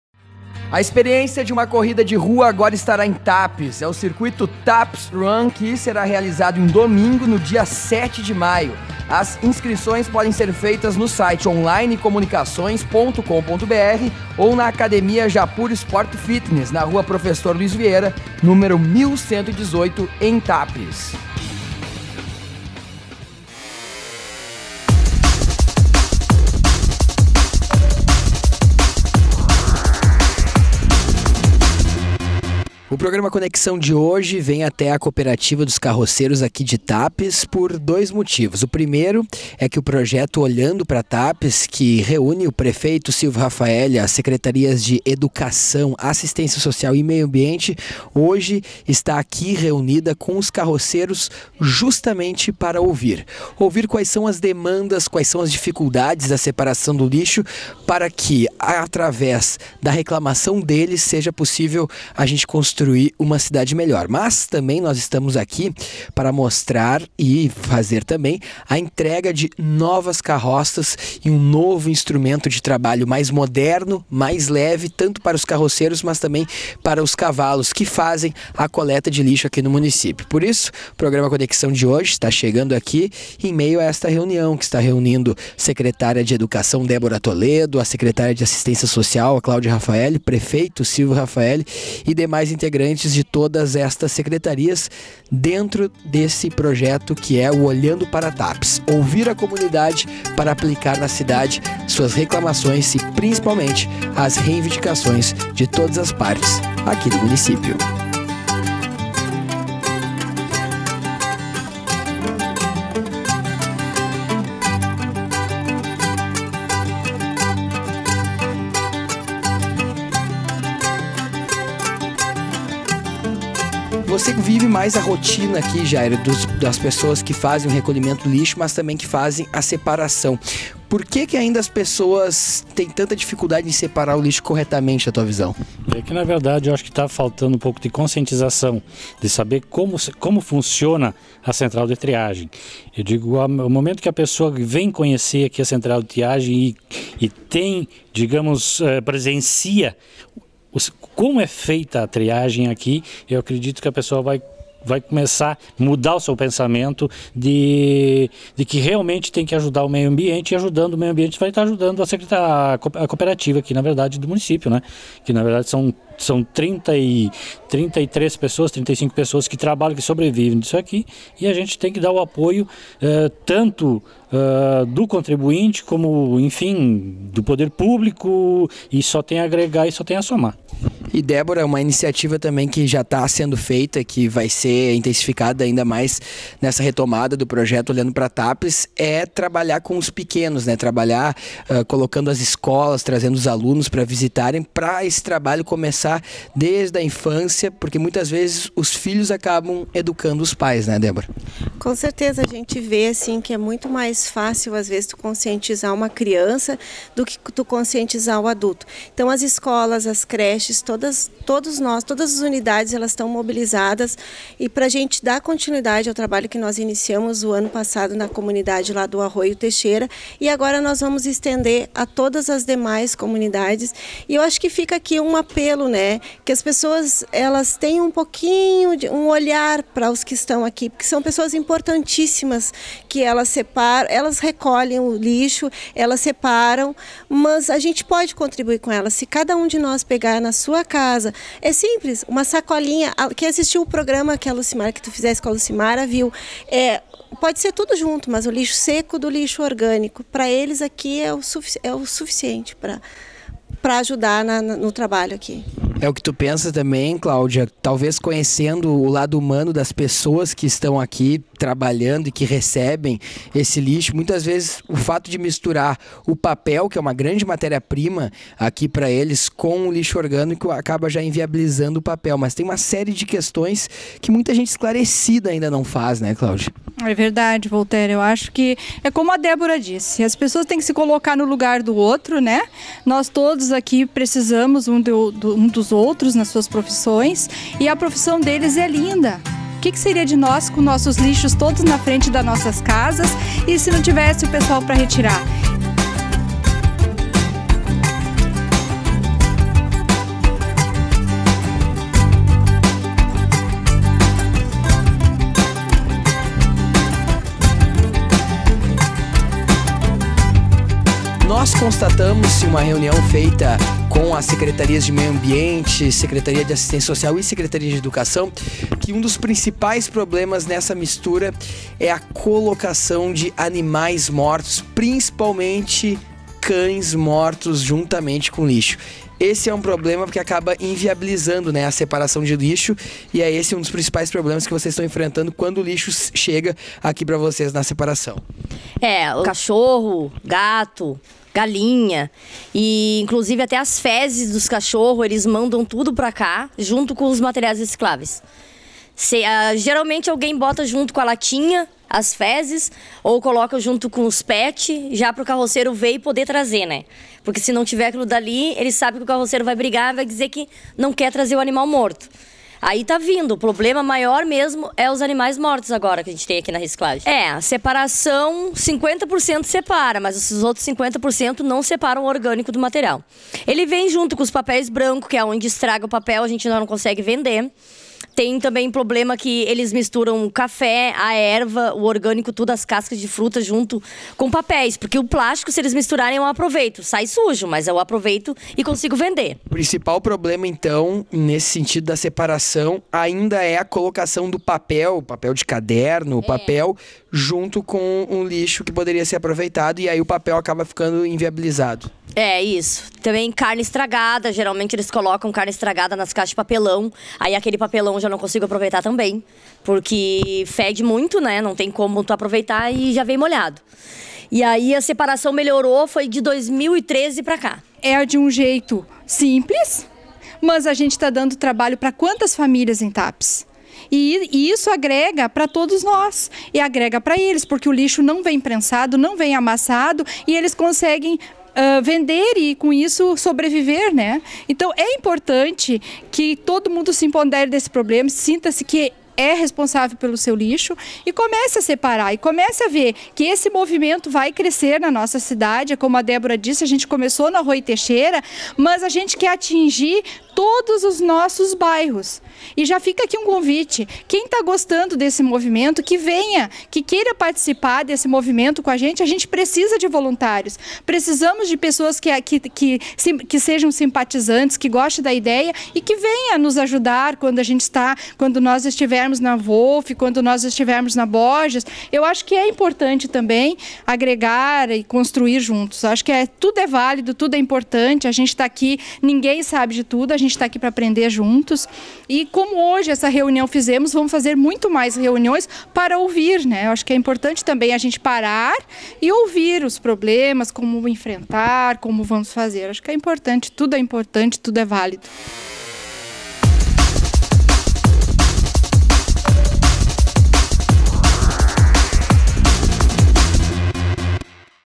CONEXÃO TAPENSE - "Projeto Olhando para Tapes" foi até a Central de Triagem de lixo.
O Programa Conexão de hoje acompanha o "Projeto Olhando para Tapes" que foi até a Central de Triagem da cidade para ouvir as demandas dos trabalhadores que atuam na reciclagem do lixo. Voltaire Santos​ conversa com recicladores pra saber de que forma a população pode construir uma cidade mais limpa e sustentável. O Projeto Olhando para Tapes integra as Secretarias de Educação, Assistência Social e Meio-Ambiente da Prefeitura.